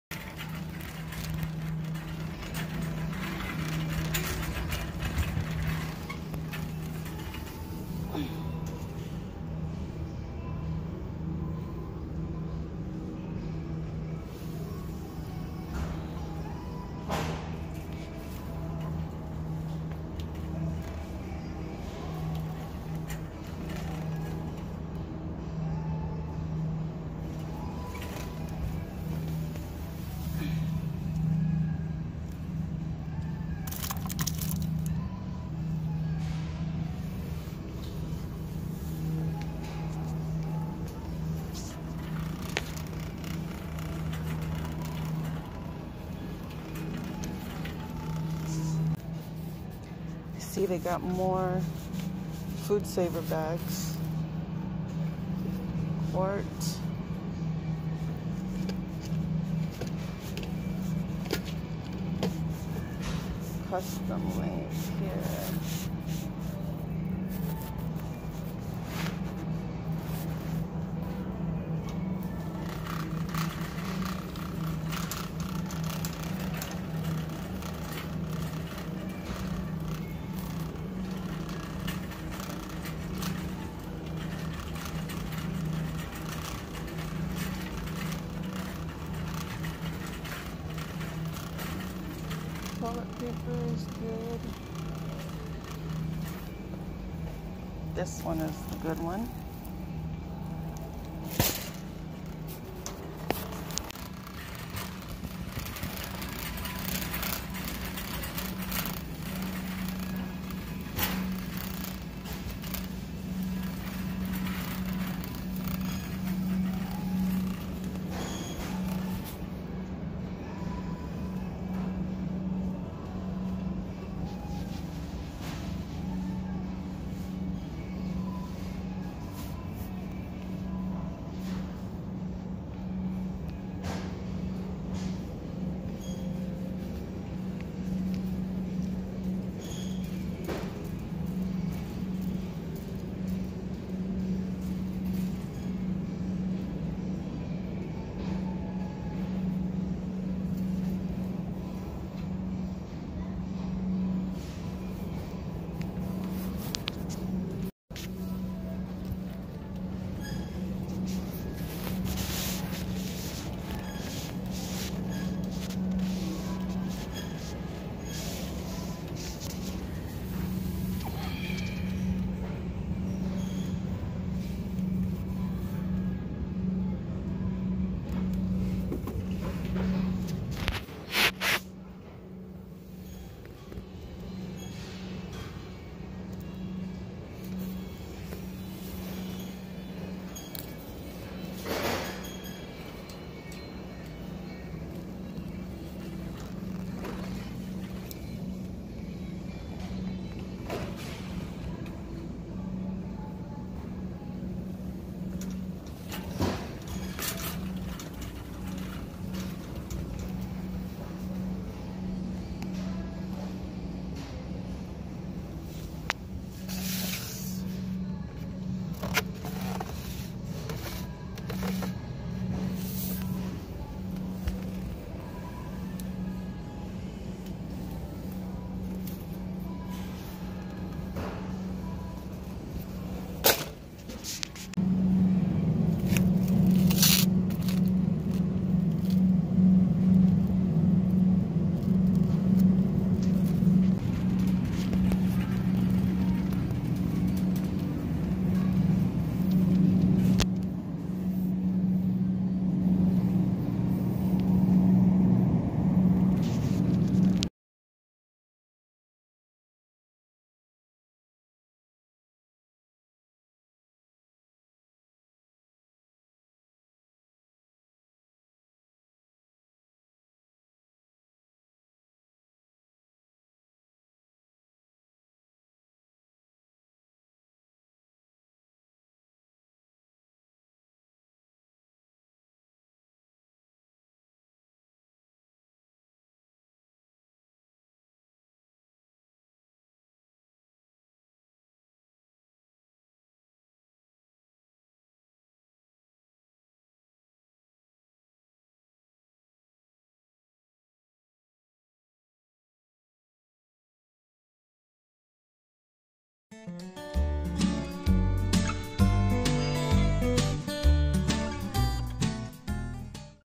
Grocery Store Walkthrough 2020 || Save Mart Shelves
I popped into the store to pick up a few items that are on sale.

A friend wanted to see how certain products in the store were doing , as far as availability. I also, was curious.